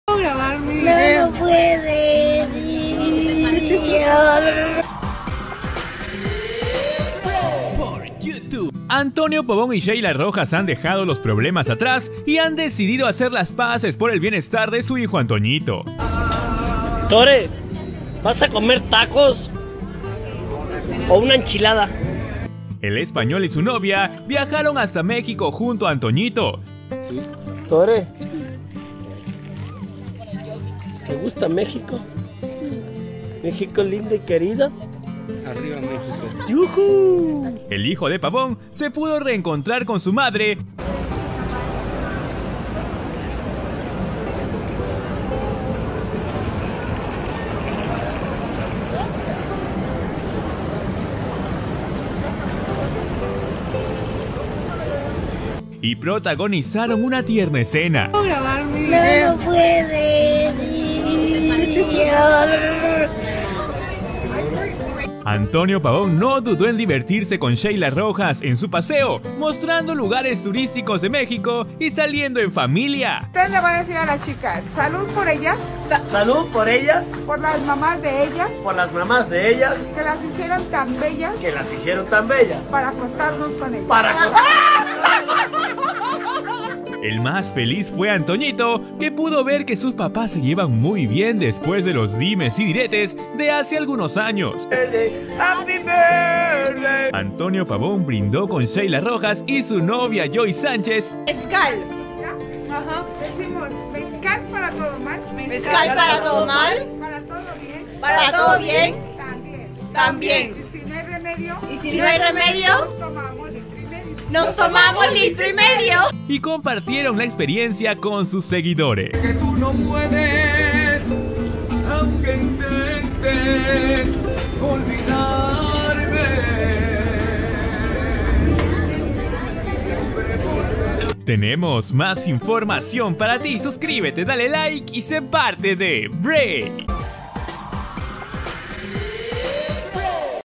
SHEYLA ROJAS Y ANTONIO PAVÓN SE REENCONTRARON EN MÉXICO JUNTO A SU HIJO ANTOÑITO
Sheyla Rojas y Antonio Pavón se vieron las caras en México y se divierten junto a sus hijos.